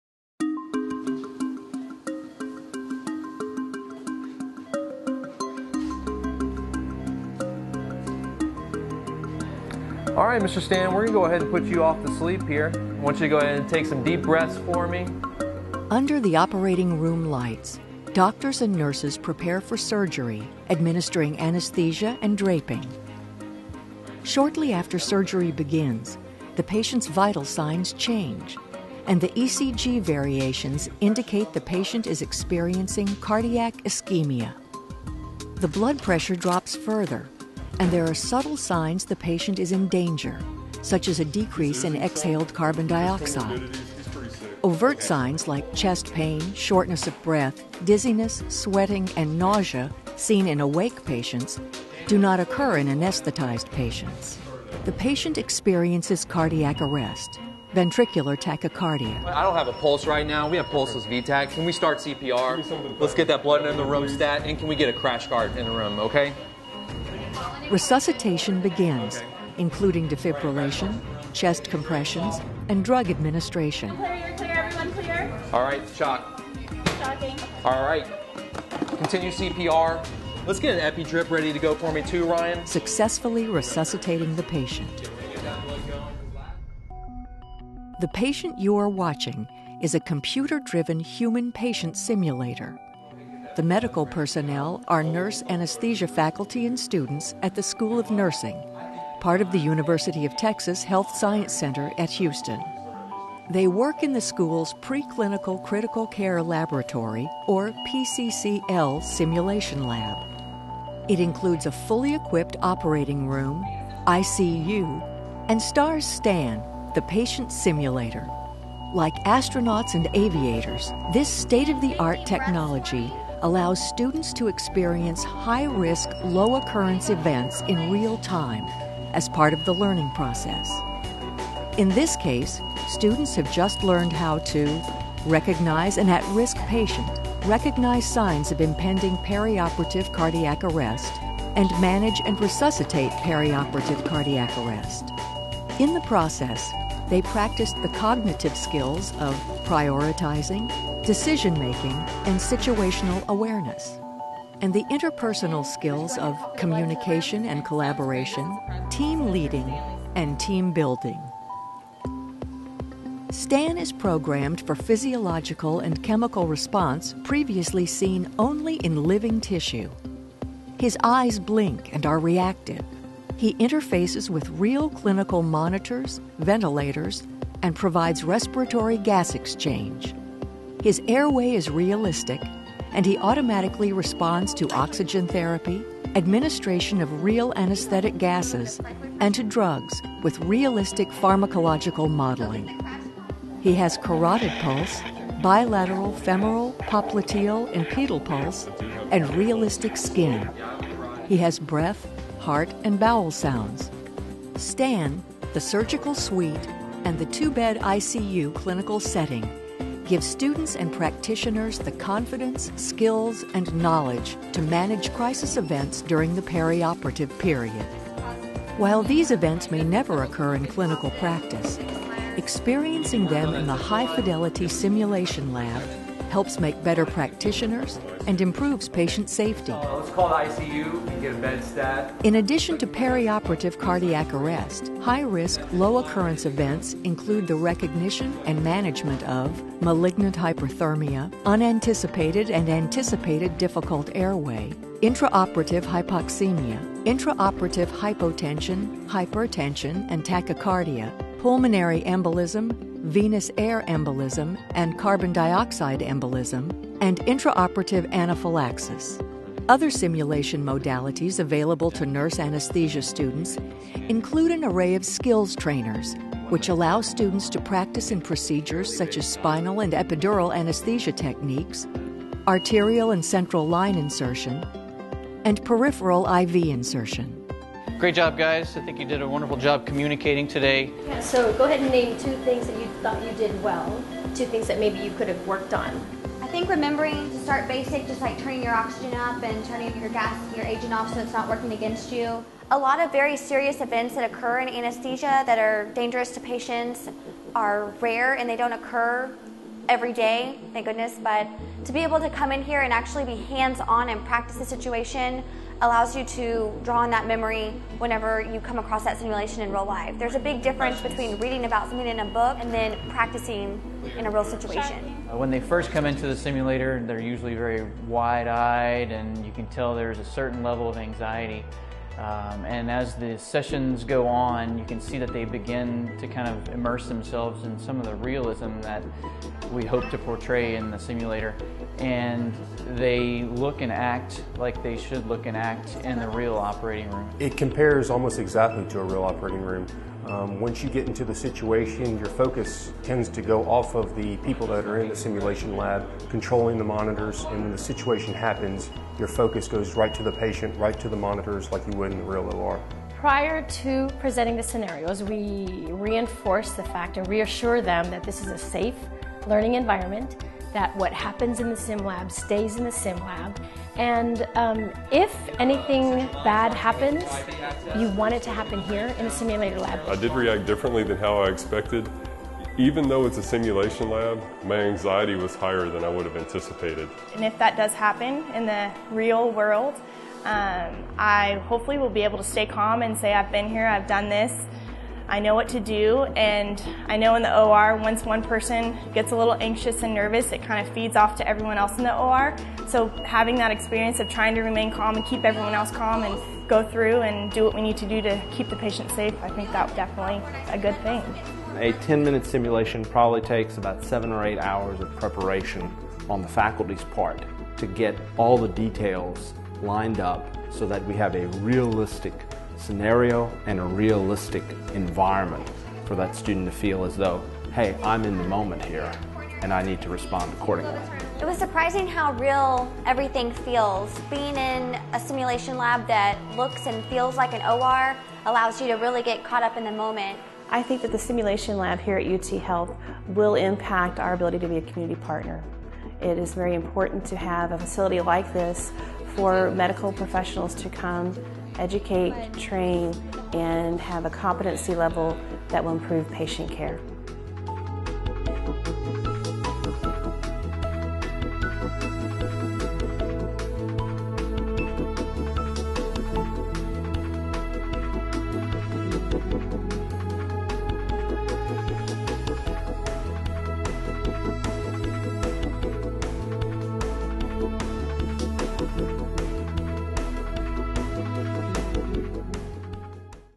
UTHealth School of Nursing SIM Lab
This high-definition video, “21st Century Simulation Innovation in Education & Research,” dramatically demonstrates how a top-tier training center at The University of Texas Health Science Center at Houston (UTHealth) School of Nursing simulates "nearly real-life" conditions.  The Pre-clinical Critical Care Laboratory (PCCL) allows nursing students to experience unavoidable high-risk situations in a clinical setting and learn hands-on skills for making patients as safe as possible. This video received the Award of Merit-Best Shorts Competition in 2012.